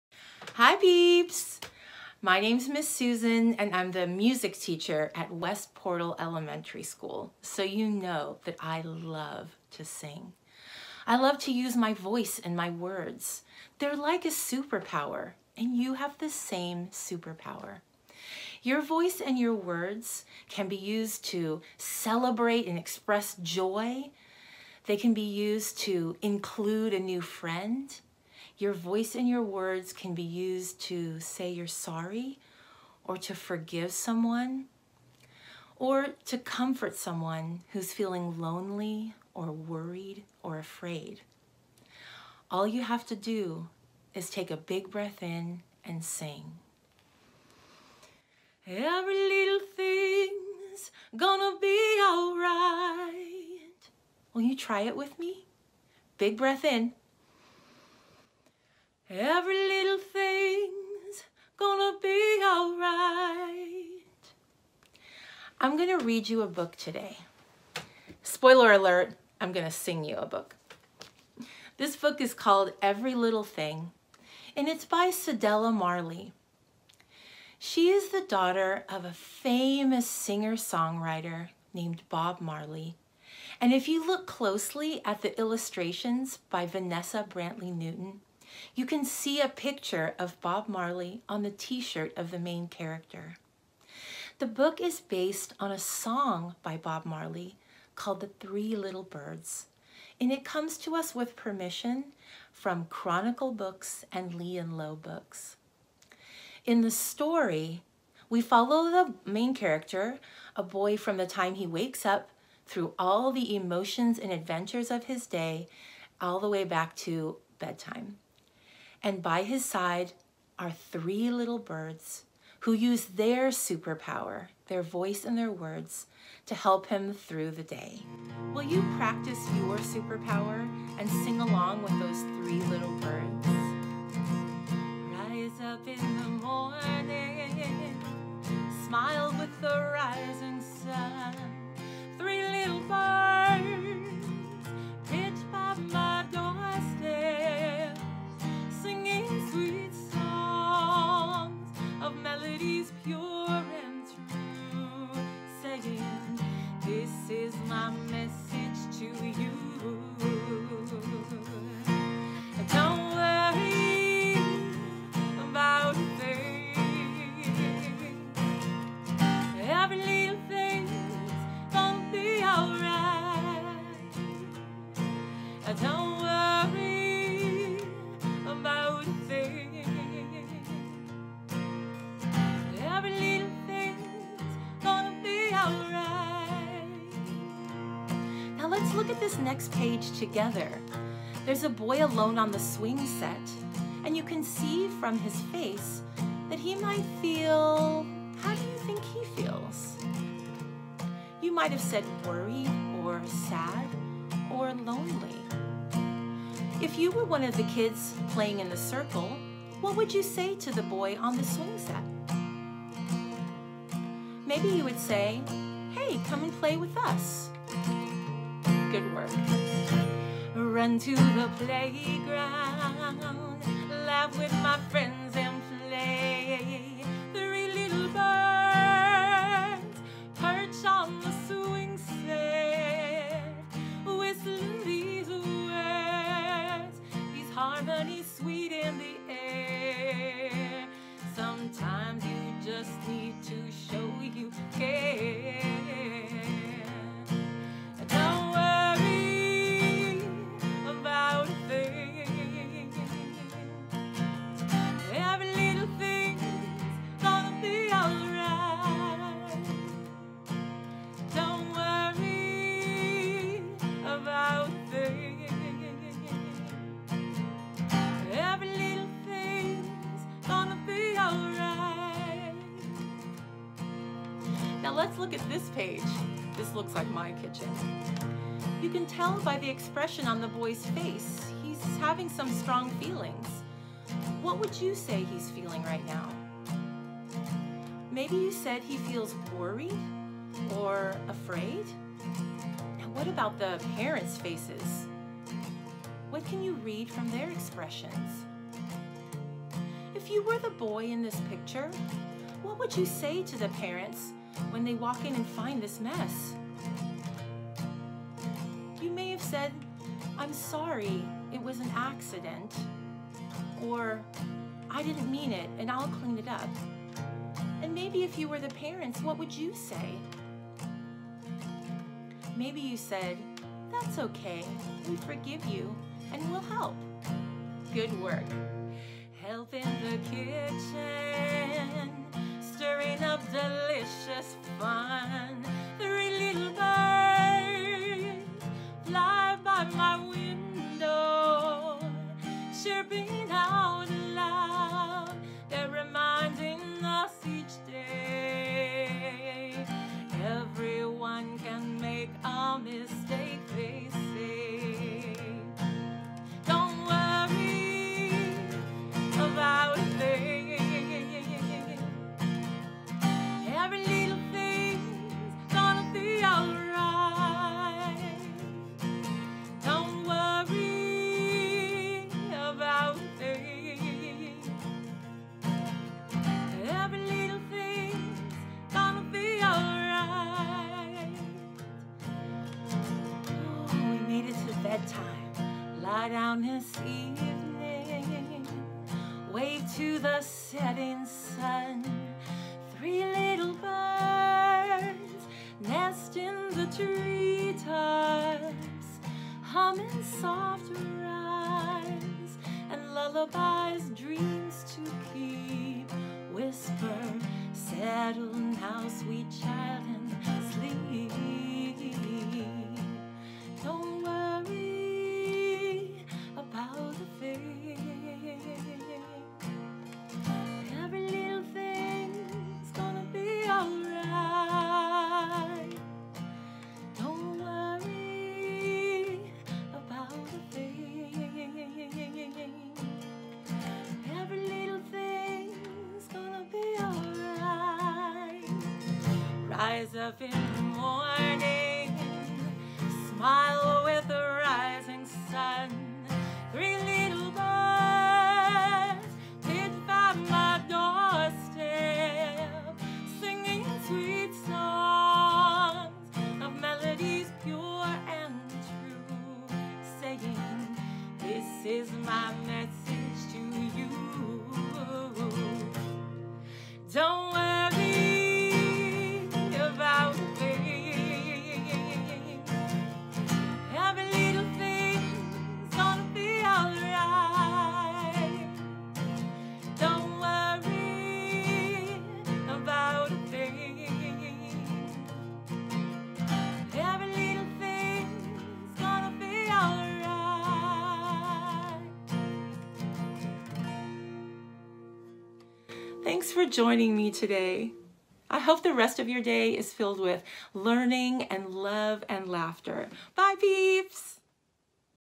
Every Little Thing
This upbeat story reminds children that the sun will always come out after the rain and mistakes are easily forgiven with a hug. Every family will relate to this universal story of one boy who won't let anything get him down, as long as he has the help of three very special little birds. 03/25/2021